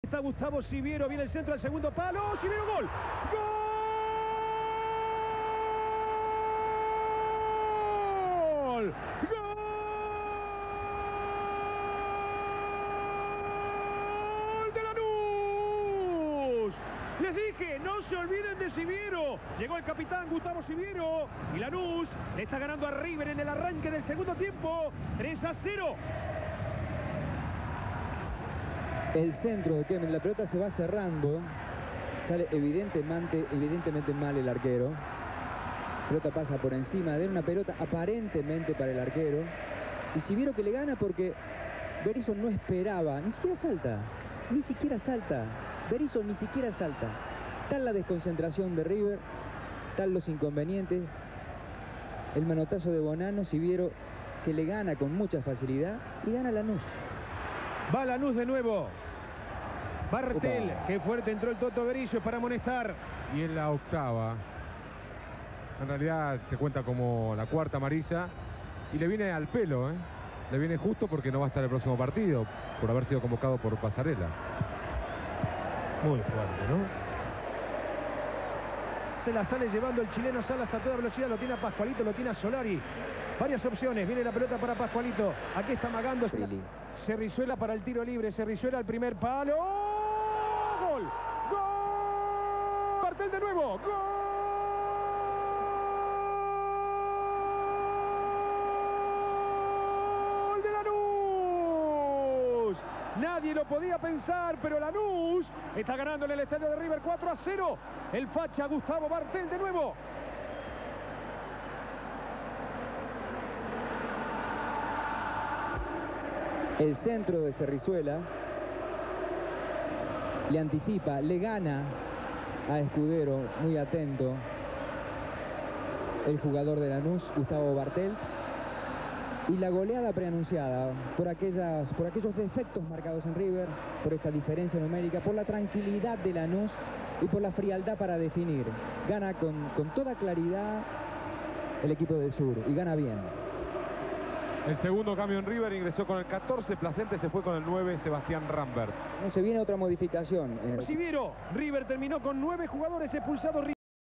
16-2-1998 (Clausura) (1°F) River Plate:0 vs Lanus:4 (2° Parte)
Goels:Siviero-Bartelt.